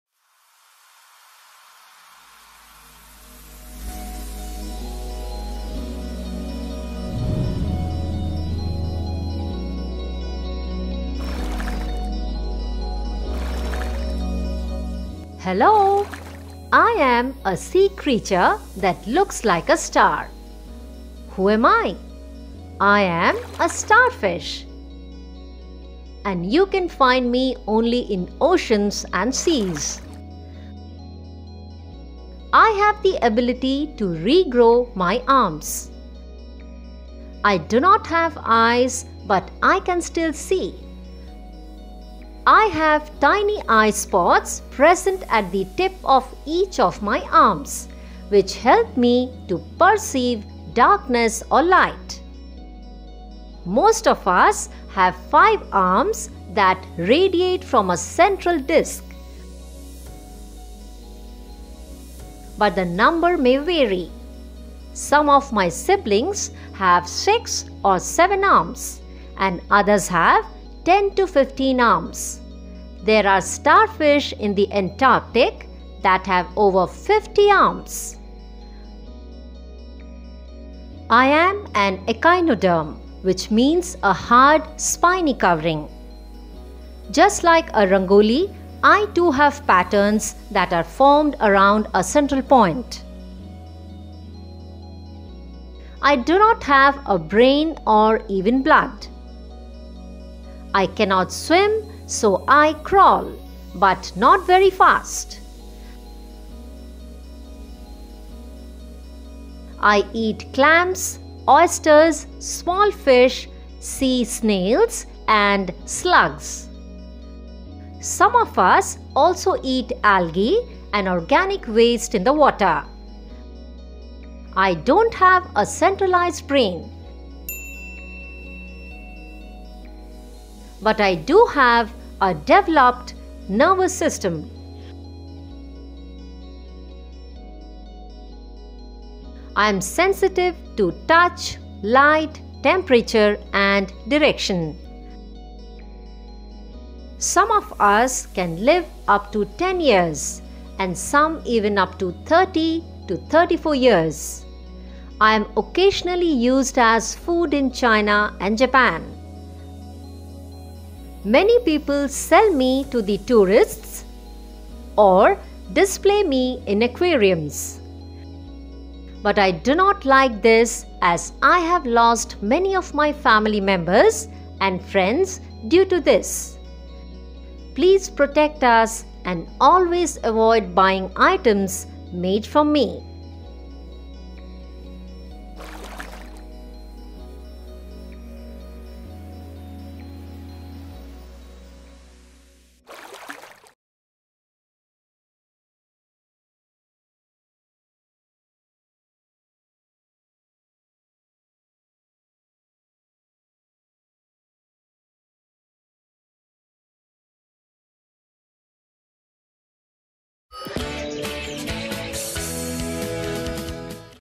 Starfish
"As much of the Earth’s surface is underwater, it comes as no surprise that marine species outnumber those on land. Scientists estimate that about one million species of animals live in the ocean. There's no doubt that the world's oceans and rivers are home to incredible creatures.

Let's dive in and explore this amazing and beautiful life of Starfish underwater."